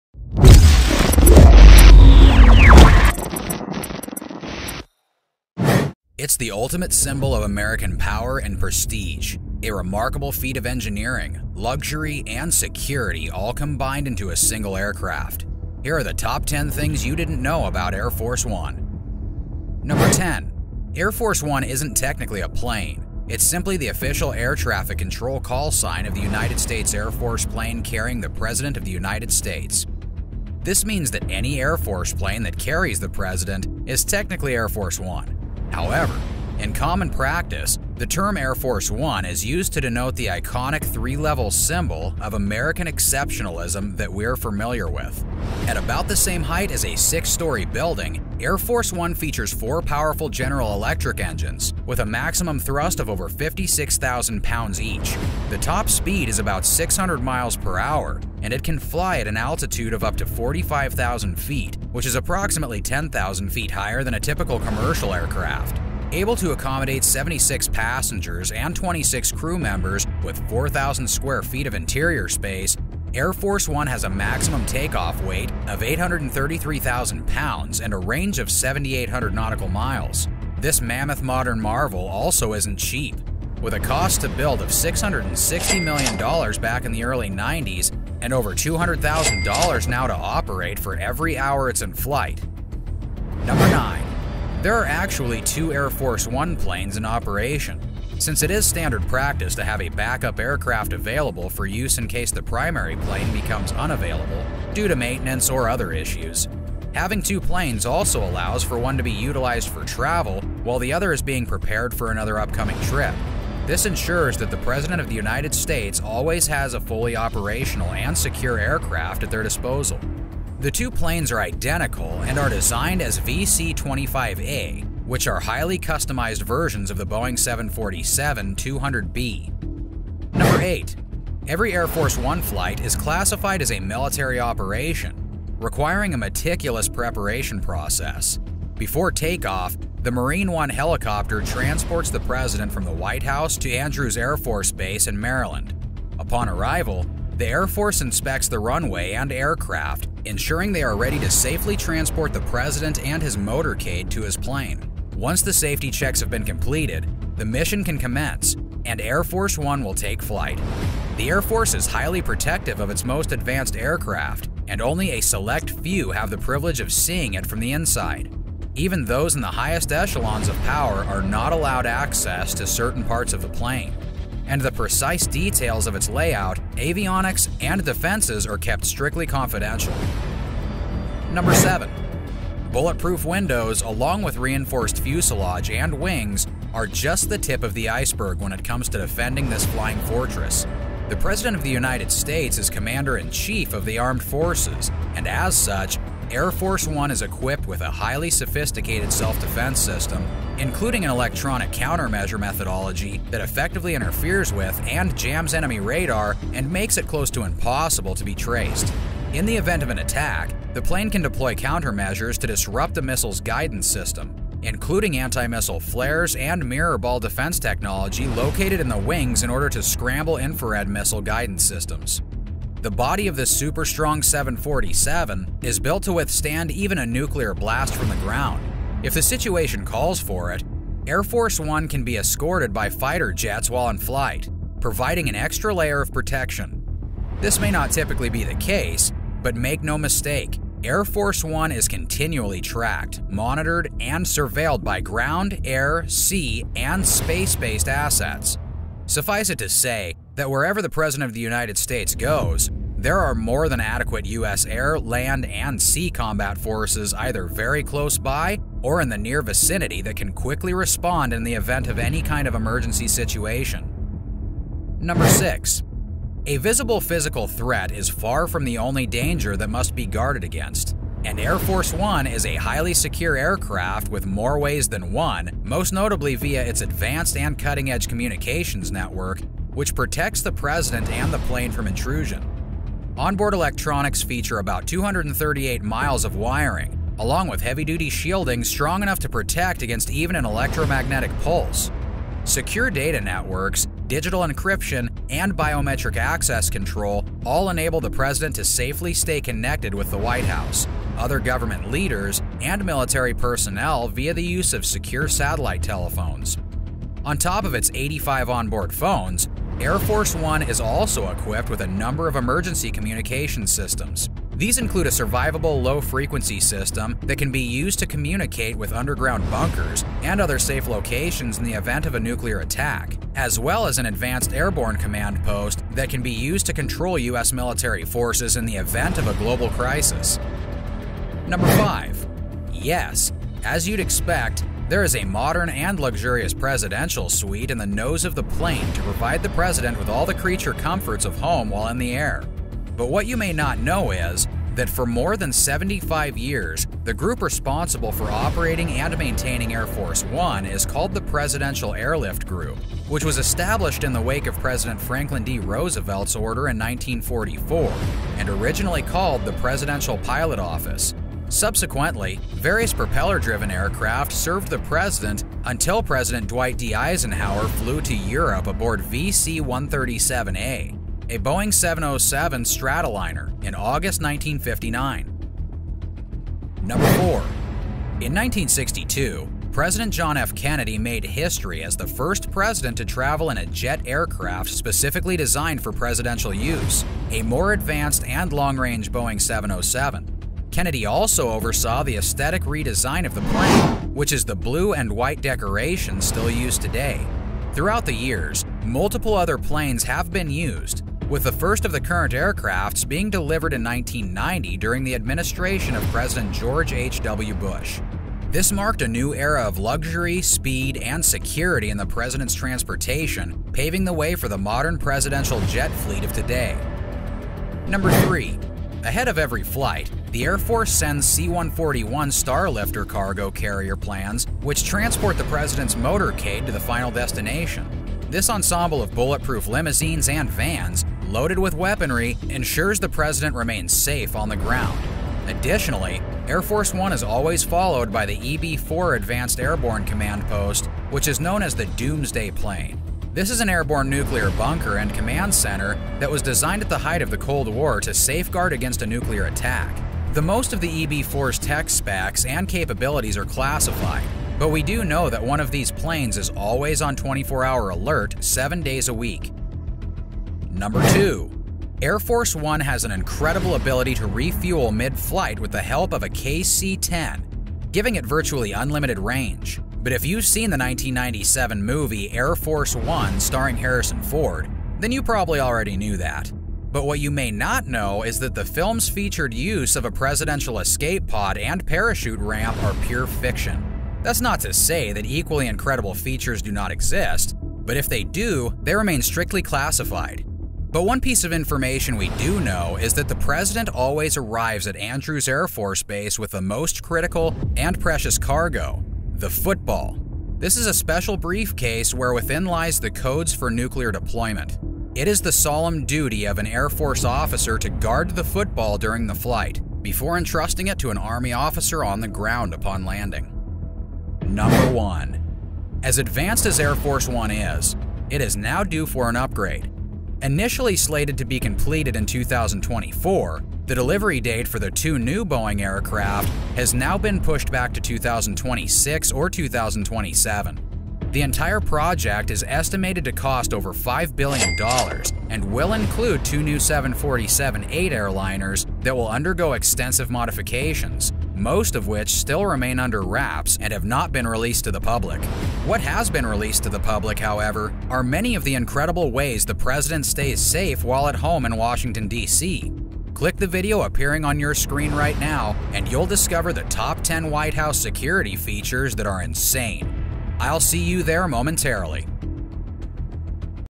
10 Things You Didn't Know About Air Force One
It’s the ultimate symbol of American power and prestige...

A remarkable feat of engineering, luxury, and security all combined into a single aircraft.

Here are the top 10 things you didn’t know about Air Force One.